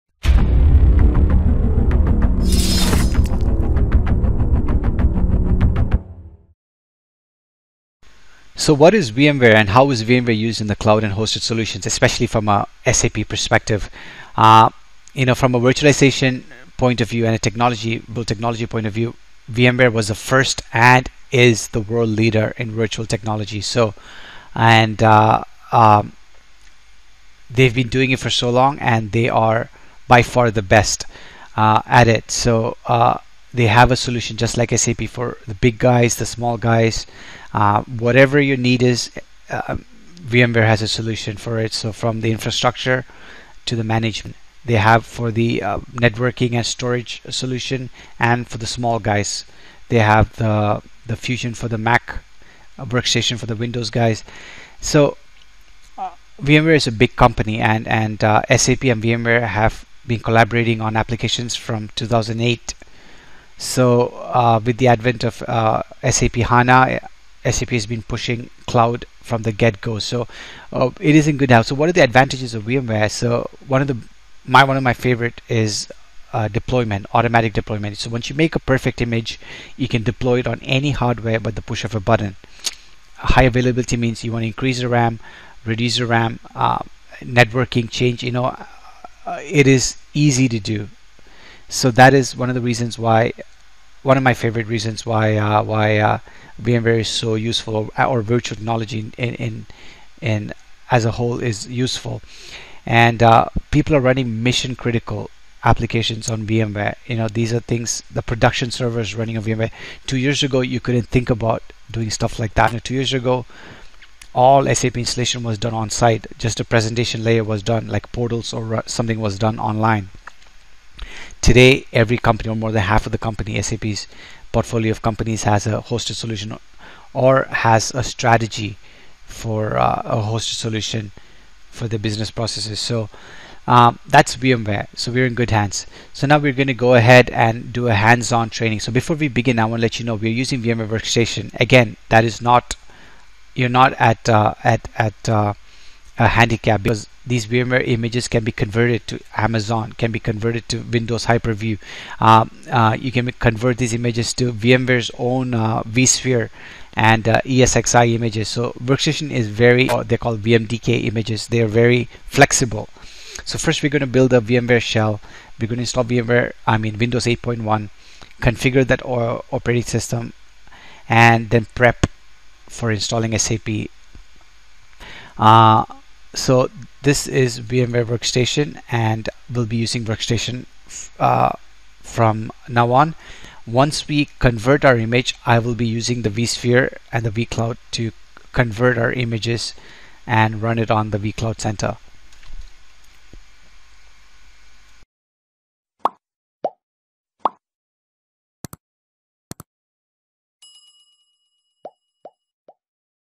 1- "Why Choose VMware for SAP BASIS? Key Advantages Explained"
This session explores why VMware is an ideal choice for SAP BASIS environments. It covers key benefits like enhanced flexibility, improved system performance, and simplified maintenance. Learn how VMware’s virtualization capabilities enable better resource management, scalability, and cost-efficiency, making it a valuable tool for running and managing SAP applications. This overview will help SAP BASIS administrators and IT managers understand how VMware can streamline operations and optimize SAP infrastructure.